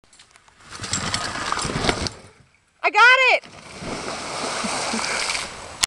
Derek Backflip on Bike
Derek backflips off ramp into pond